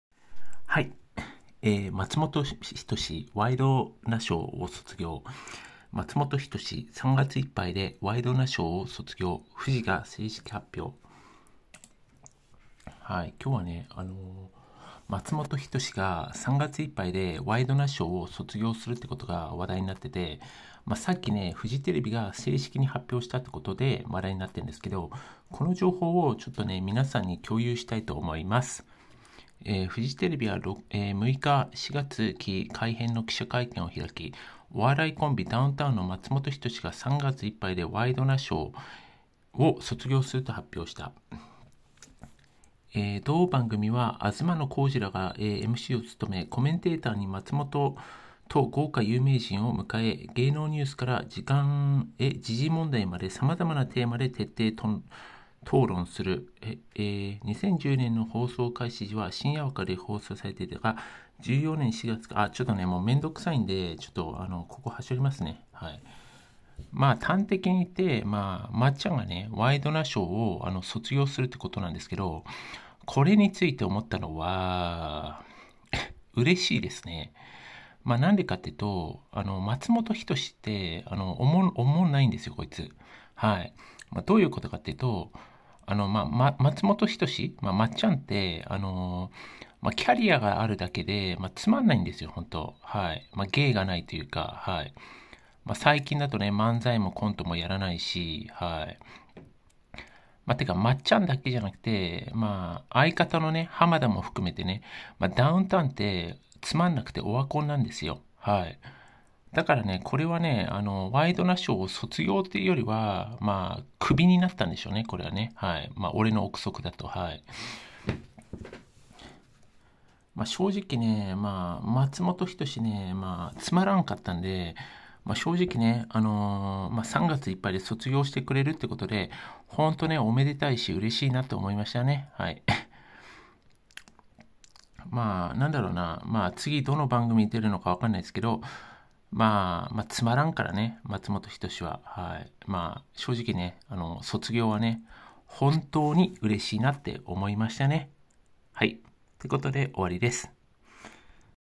松本人志さん ワイドナショー 卒業 に批判殺到。許せない (ワイドナ フジテレビ)
松本人志 ワイドナショーを卒業
松本人志、3月いっぱいで『ワイドナショー』卒業　フジが正式発表

フジテレビは6日、4月期改編の記者会見を開き、お笑いコンビ・ダウンタウンの松本人志が3月いっぱいで『ワイドナショー』（毎週日曜　前10：00）を卒業すると発表した。

　同番組は東野幸治らがMCを務め、コメンテーターに松本と豪華有名人を迎え、芸能ニュースから時事問題まで、さまざまなテーマで徹底討論する。2013年の放送開始時は深夜枠で放送されていたが、14年4月から日曜午前に異動した。松本は、同番組開始時からレギュラー出演していたが、22年4月から隔週での出演となっている。

　松本は中居正広とともに、4月から始まるトーク＆パフォーマンス番組『まつもtoなかい』（毎週日曜　後9：00）に出演すると今回の会見で発表された。松本は出演発表にあたりコメントを寄せ「『まつもtoなかい』は『ワイドナショー』を辞めてまでの意気込みで臨む、ということです」と心境を明かしている。

　編成制作局編成センター局長職兼室長兼編成部長の中村百合子氏は「松本さん隔週でご出演していただいているが、3月いっぱいでご卒業になります」と説明した。卒業の体制について「いろんなフォーメーションで、今までの番組のコンセプトそのままに刺激的な面白い番組を続けていければ」とした。

　同局の春の改編テーマは「楽しいで　ぬりかえろ！～フジテレビスピリット～」。改編率は全日28.8％、ゴールデン帯27.6％、プライム帯30.4％となった。